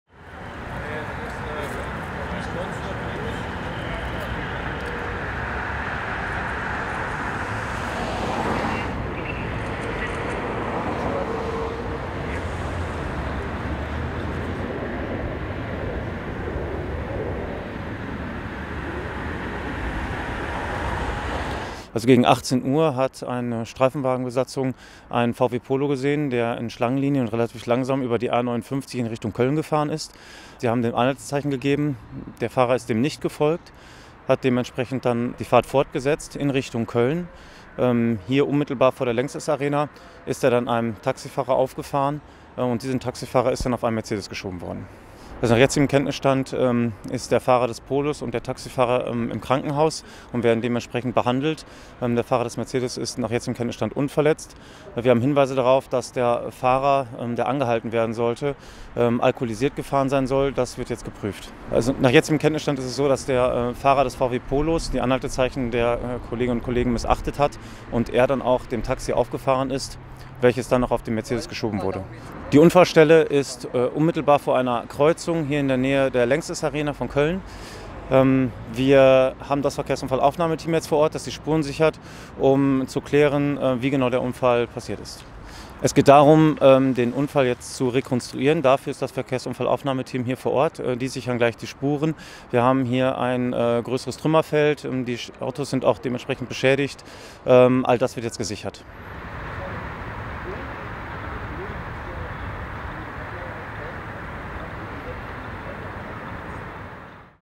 Besoffener Autofahrer flüchtet vor Polizei und verursacht schweren Unfall in Köln-Deutz | 28.09.2024
Am Samstagabend (28. September) ist ein stark alkoholisierter Mann (67) vor der Lanxess-Arena mit seinem VW Polo auf einen an einer Ampel stehendes Taxi (Fahrer: 30) aufgefahren. Durch die Wucht des Aufpralls wurde das Taxi auf einen ebenfalls wartenden Mercedes (Fahrer: 38) geschoben. Der 67-Jährige und der Taxifahrer erlitten schwere Verletzungen und werden derzeit stationär in einer Klinik behandelt. Dort wurde dem Unfallverursacher, bei dem ein Atemalkoholvortest rund 1,4 Promille ergeben hatte, eine Blutprobe entnommen. Den schwerbeschädigten VW Polo stellten die Polizisten sicher. Das Verkehrsunfallaufnahmeteam sichert aktuell die Spuren. Für die Zeit der Unfallaufnahme ist der Östliche Zubringer stadteinwärts gesperrt. Der Verkehr wird über die Severinsbrücke abgeleitet.

Der 67-Jährige war Polizisten gegen 18 Uhr auf der BAB 559 in Fahrtrichtung Köln aufgefallen, da er in Schlangenlinien fuhr. Er hatte Anhaltezeichen missachtet und stattdessen beschleunigt.

Quelle: Polizei Köln
Bericht: Daniel Evers
Kamera: Canon XA75